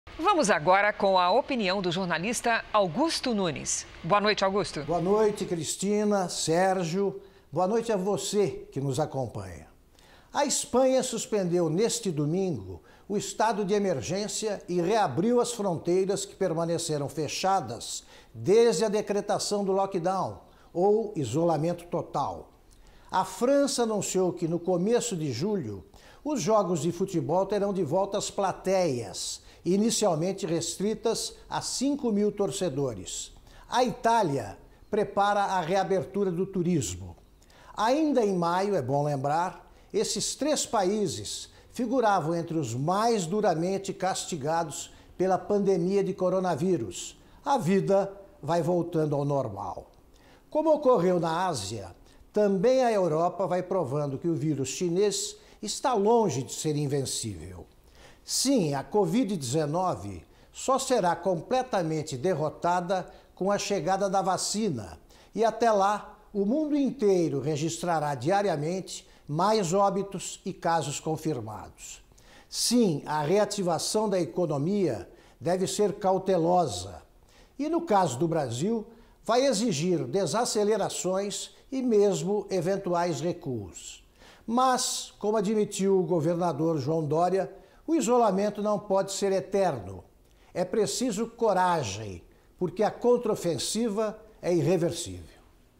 Augusto Nunes: países da Europa retomam a vida mesmo com a pandemia
O jornalista Augusto Nunes comenta a volta à normalidade de alguns países. A Espanha suspendeu o estado de emergência e reabriu as fronteiras neste domingo (21). A França anunciou a volta das torcidas nos jogos de futebol a partir de julho. E a Itália prepara a reabertura do turismo. Esses três países, até o mês passado, figuravam entre os territórios mais castigados pela pandemia coronavírus, mas, agora, eles veem a vida voltando ao normal.